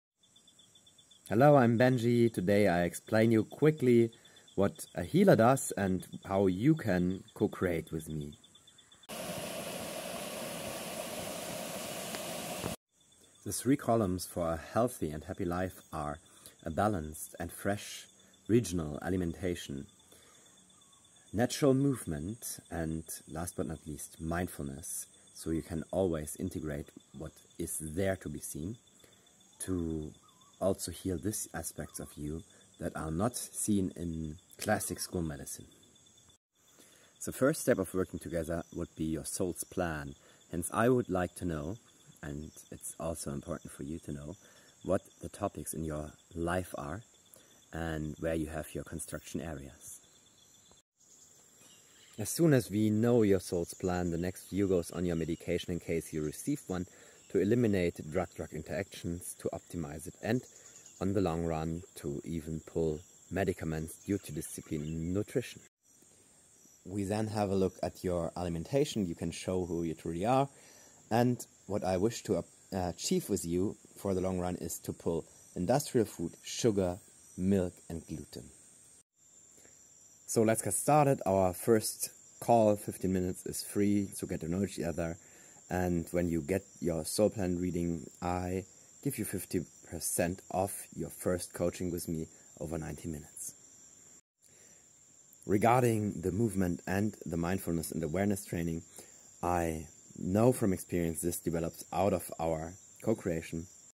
The Traveling Wayshower. That's how our cocreation could look like.
In the video I suggest, how we can cocreate to unfold your most beautiful Life. For me this is getting to know the three most important ingredients for a happy Life, that would be the nutrition, movement and mental health. This is quite a personal thing and I can guide you on your way to a holistic Life. Reach out for more information. You find me via the following websites.

Looking forward to Cocreation with you. Thank You!